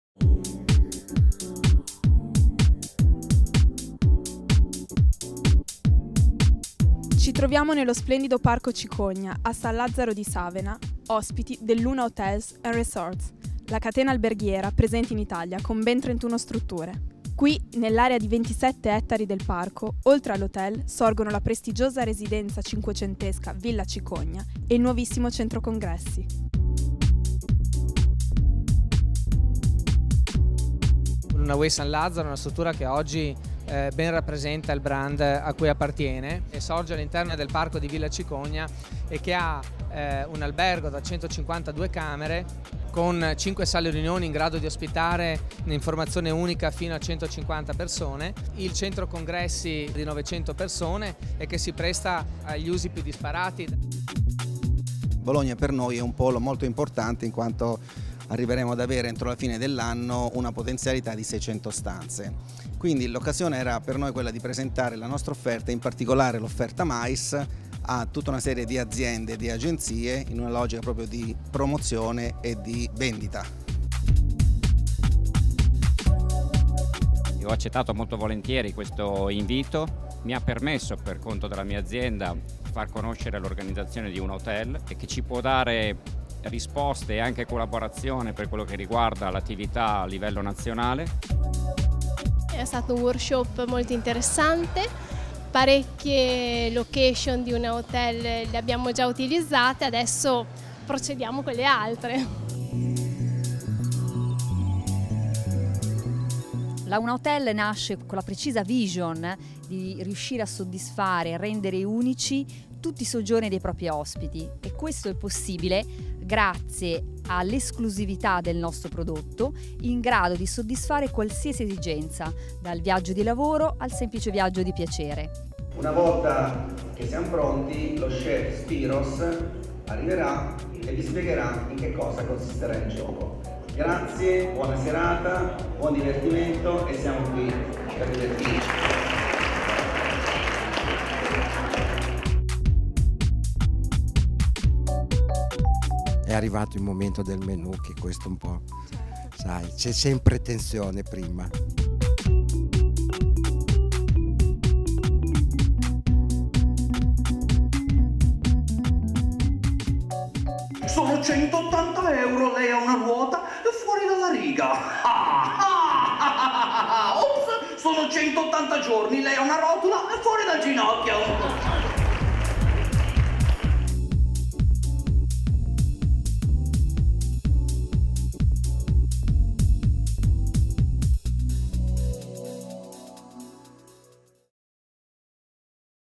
Unaway Hotel Workshop Bologna - Villa Cicogna San Lazzaro
Documentazione video con interviste per la presentazione del nuovo Hotel.
UNA Hotels & Resorts nasce nel novembre 2000 come progetto mirato a costituire un nuovo polo alberghiero italiano basato su solide competenze manageriali.